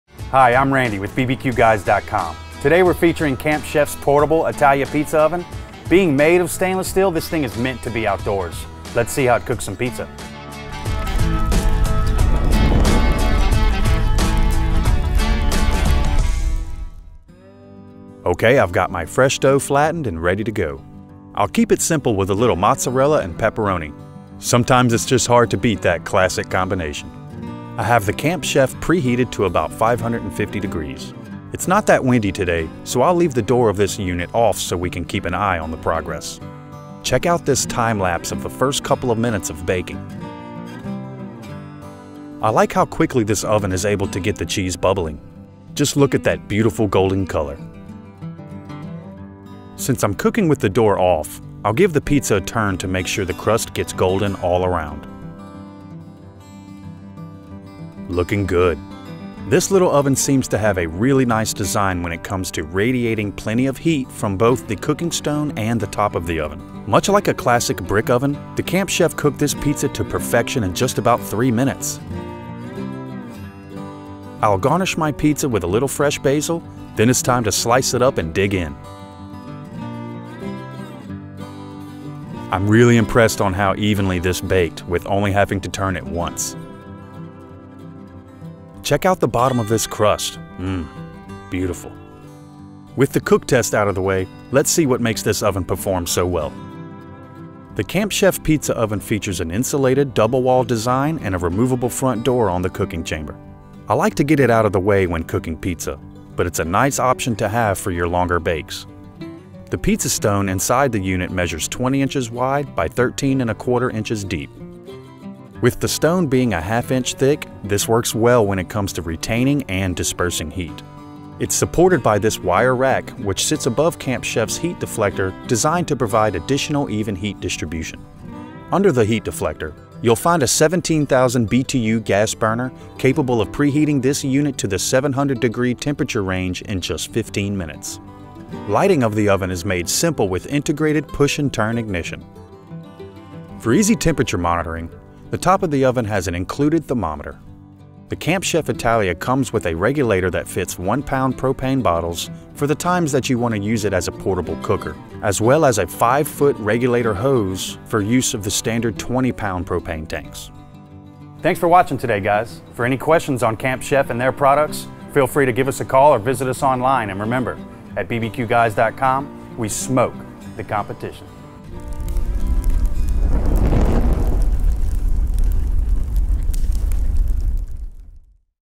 Camp Chef Italia Artisan Tabletop Gas Pizza Oven Review | BBQGuys.com
Take a look at how the Camp Chef Pizza Oven cooks some pepperoni pizza in this overview!

About the Camp Chef Pizza Oven:
- Powerful burner preheats and is ready for cooking in 15 minutes
- Stainless steel construction is durable and easy to clean
- Portable design is perfect for camping, tailgating or backyard cooking
- Runs on a one pound disposable propane bottle or adapts to standard size propane tank
     Add a taste of Italy to your outdoor cooking with this Camp Chef Italia Artisan portable pizza oven. This portable pizza oven is designed to replicate the performance of a wood-fired brick oven while providing the heat control and responsiveness to cook the perfect pizza every time. Featuring a specially designed 17,000 BTU gas burner and heat plates, you can reach temperatures of up to 750 degrees Fahrenheit. The double walled construction and ventilation enhances the heating performance of this oven while the built-in ignition and micro-adjustment valve give you complete flame control. Bake pizzas, breads and desserts on a 13 X 20 inch cordierite ceramic pizza stone and monitor cooking progress with the built-in temperature gauge. The oven preheats and is ready for cooking in 15 minutes. The ventilated oven door features a handle for easy opening and the oven is constructed from durable stainless steel which makes it easy to clean. The Italia Artisan pizza oven runs on a one pound disposable propane bottle or you can use the included regulator and five foot hose to connect to a standard propane tank. Great tasting food will always bring people together, and Camp Chef produces quality products that bring out the best in outdoor cooking.

Featuring: Randy Watson
Filmed/Produced by: Paris Frederick